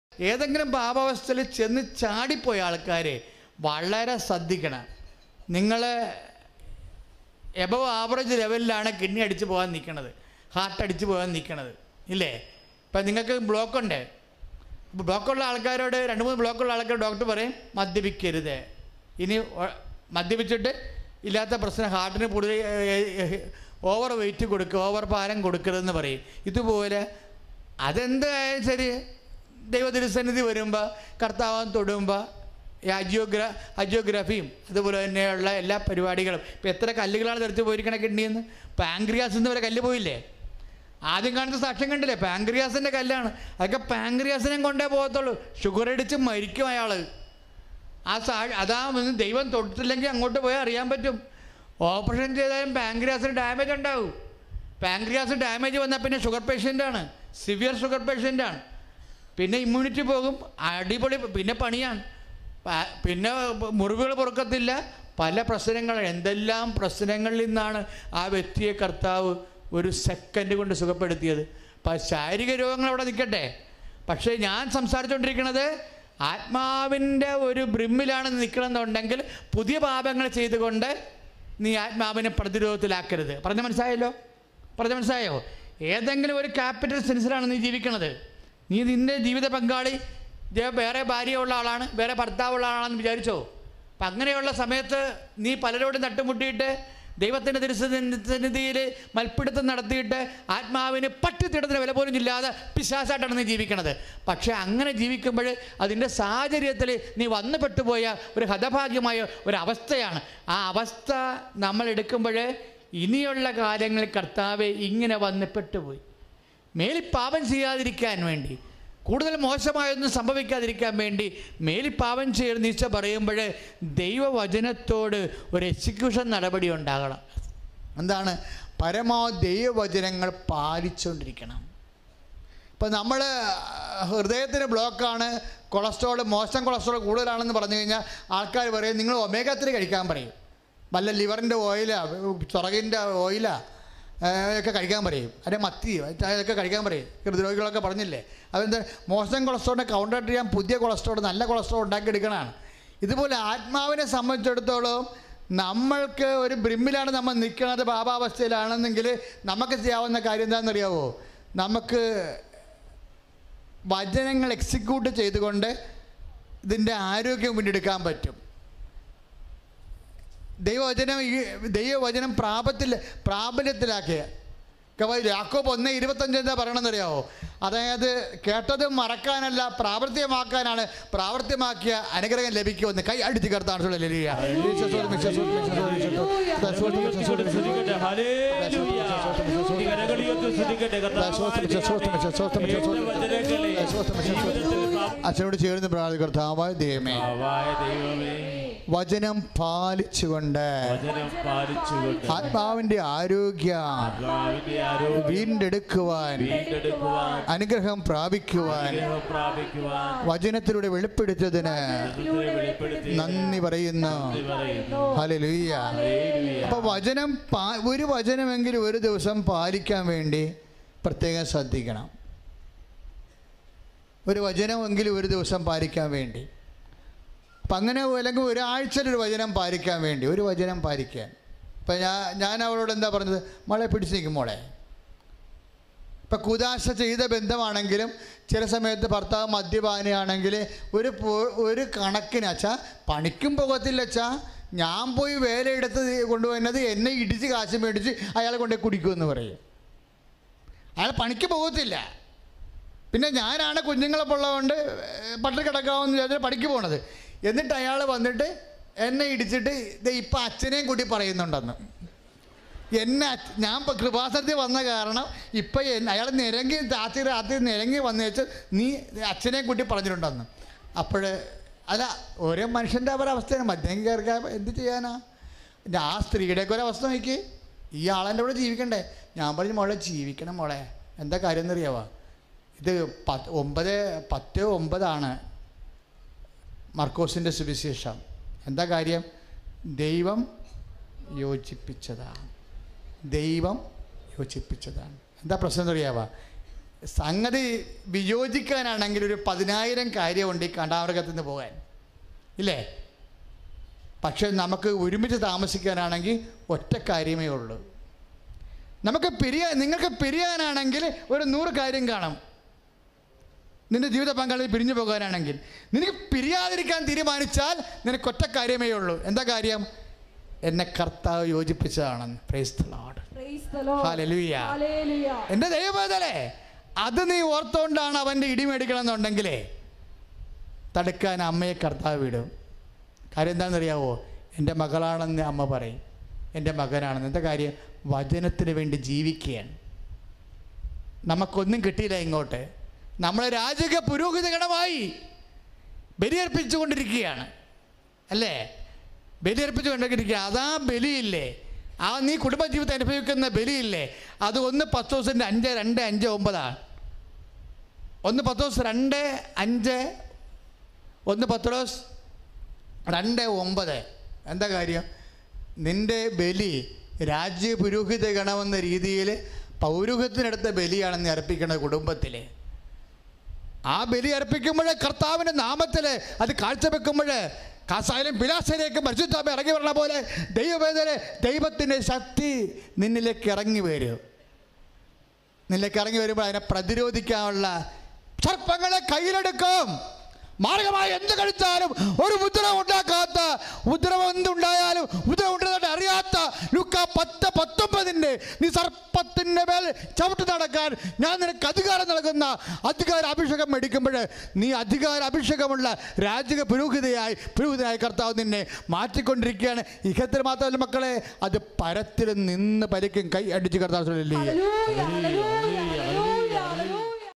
Fr V.P Joseph | വിവാഹം പിരിയാനാണെങ്കിൽ 1000 കാരണങ്ങൾ!!!!!!! പിരിയാതിരിക്കാൻ ഒറ്റ കാരണം
Fr V.P Joseph Valiyaveettil | വിവാഹം പിരിയാനാണെങ്കിൽ 1000 കാരണങ്ങൾ! പിരിയാതിരിക്കാൻ ഒറ്റ കാരണം